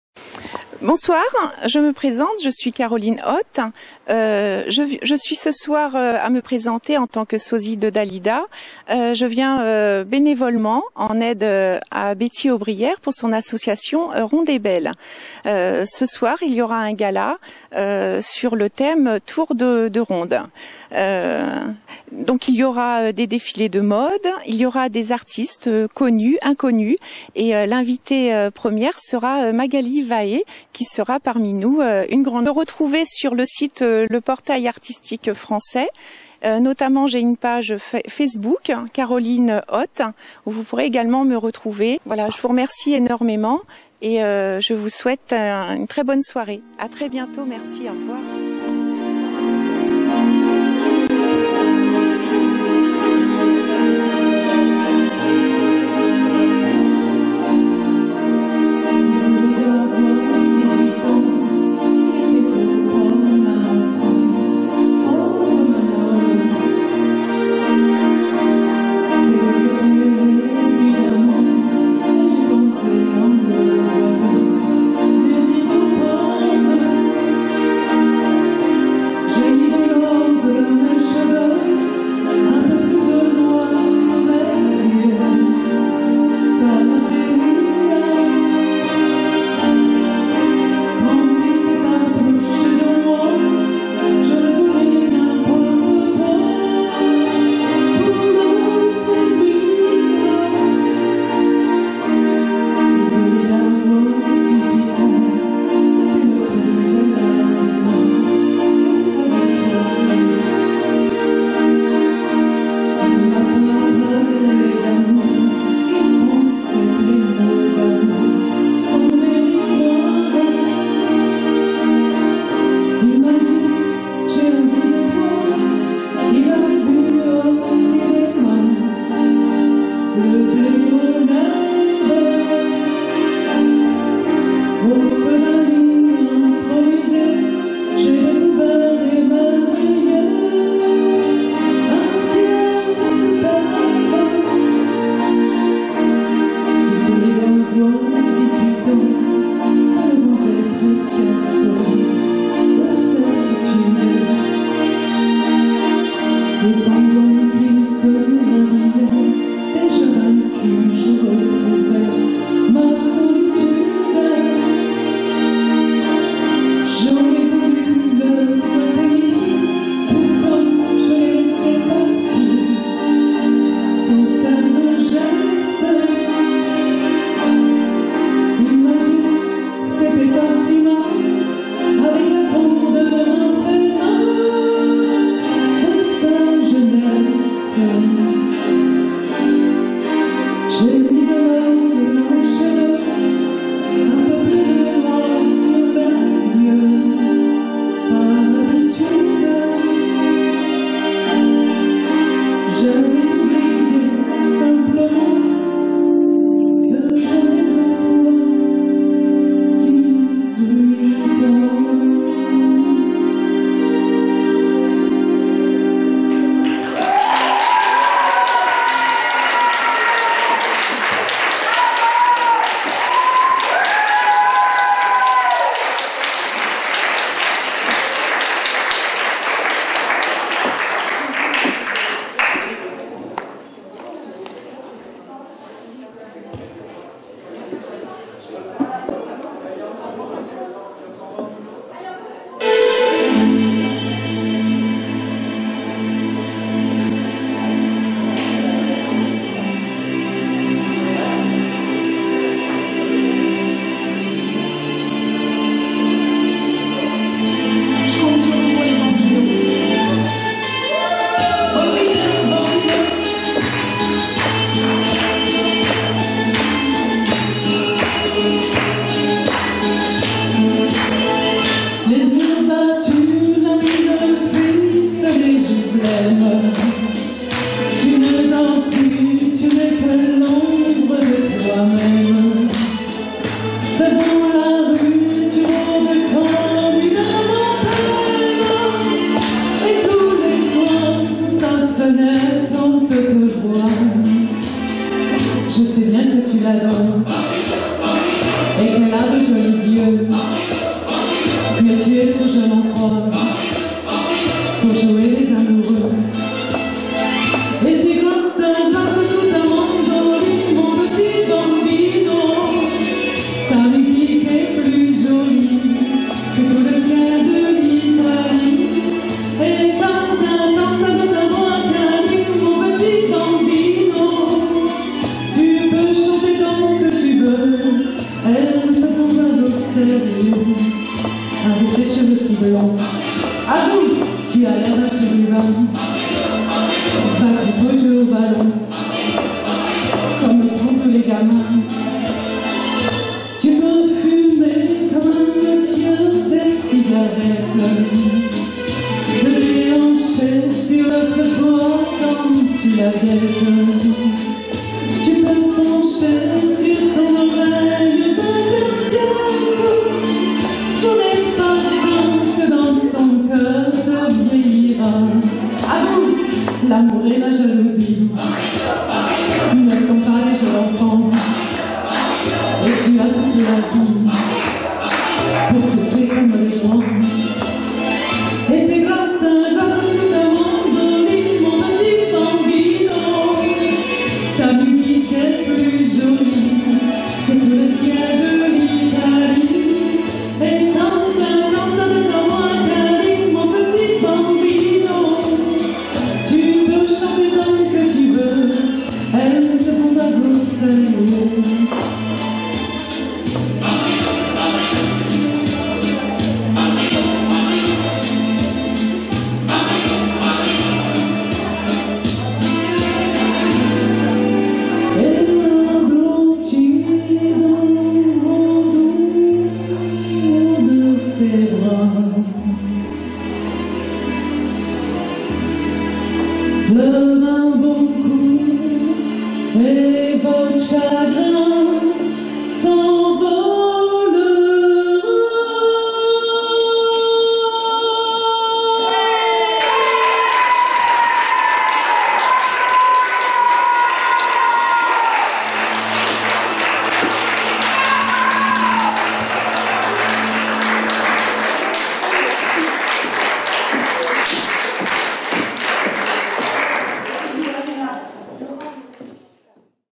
Caroline Hot
Participation de Caroline Hot qui chante Dalida lors du défilé concert à Saintes en août 2013 - Rondes et sexy.